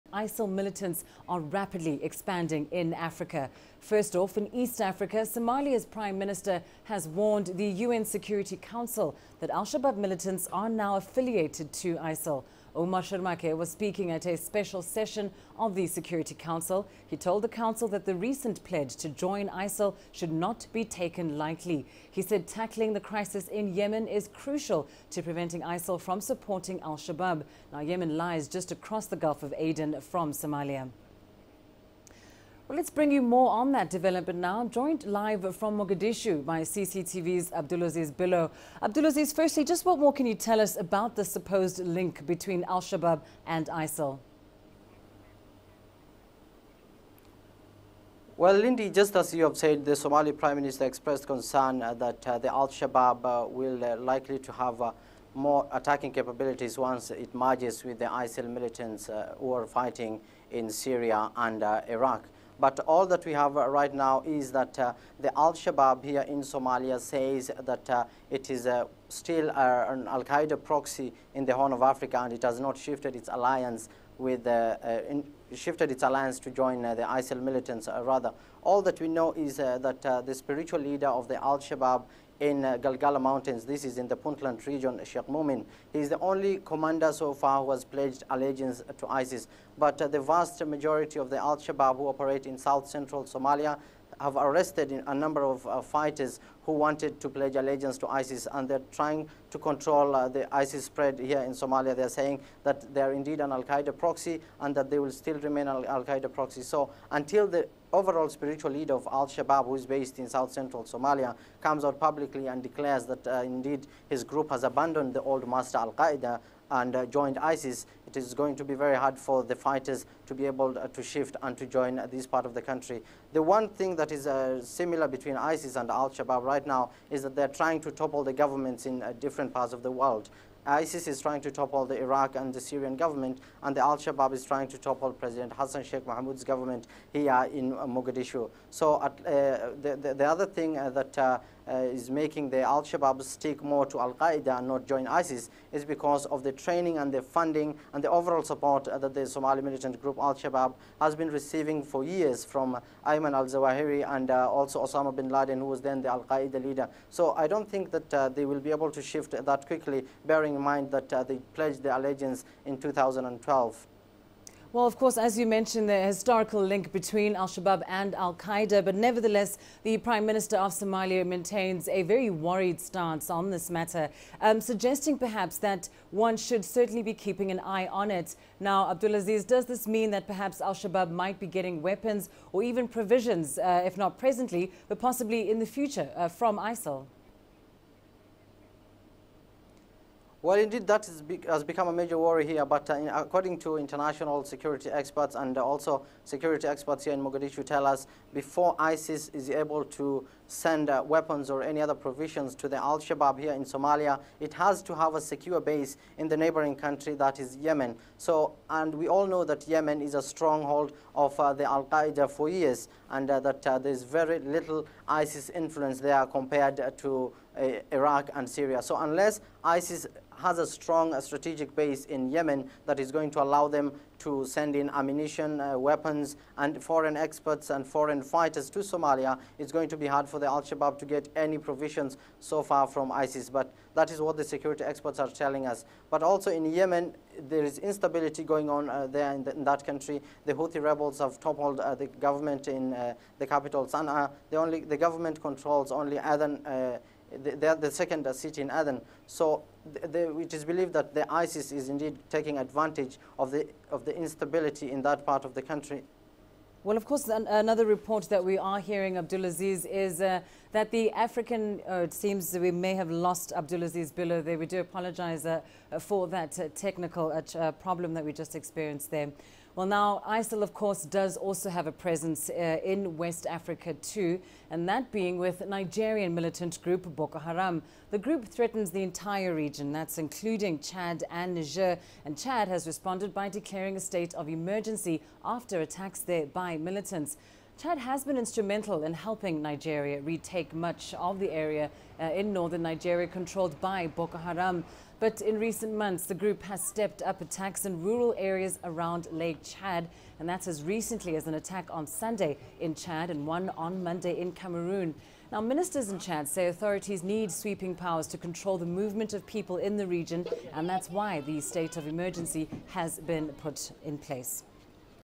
Al-Shabaab now affiliated to ISIL
Worrying reports have emerged saying that ISIL militants are rapidly expanding in Africa.
In East Africa, Somalia's Prime Minister has warned the UN Security Council that al-Shabaab militants are now affiliated to ISIL. Omar Sharmarke was speaking at a special session of the Security Council.
He told the council that the recent pledge to join ISIL should not be taken lightly, adding that tackling the crisis in Yemen is crucial in order to prevent ISIL from supporting al-Shabaab. Yemen lies just across the Gulf of Aden from Somalia.